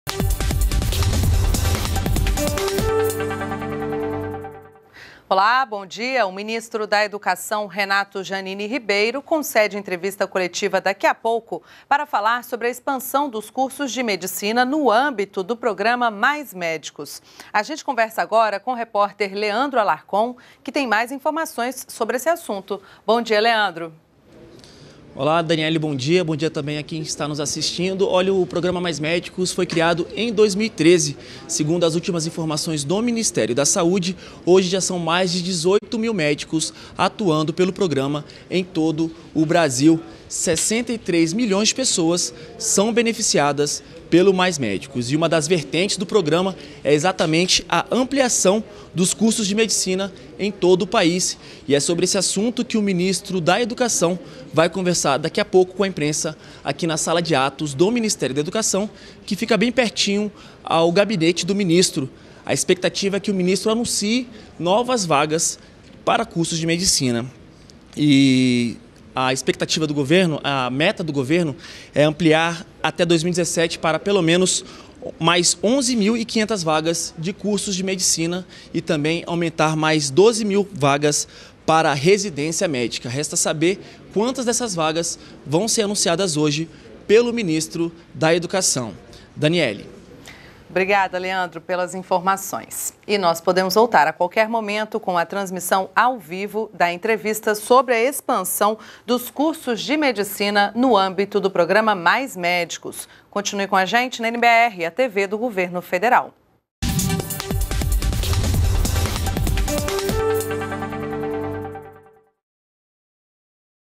Governo expandirá vagas em cursos de medicina
ATIVIDADES DO GOVERNO - 26.08.15: Nesta quarta-feira (26), o ministro da Educação, Renato Janine Ribeiro, concederá entrevista coletiva para falar sobre expansão dos cursos de medicina no âmbito do programa Mais Médicos.